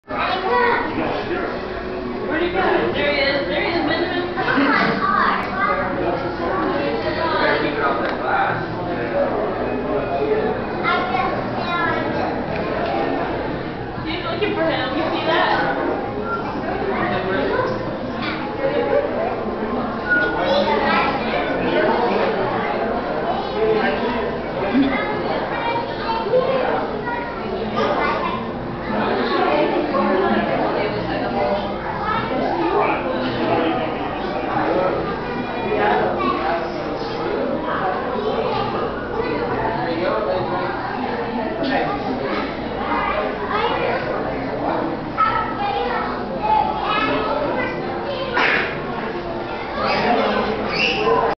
Fish plays with kids
While waiting in line for Turtle Talk with Crush at Epcot, a fish in a tank off to the side was playing with my nephew and some other kids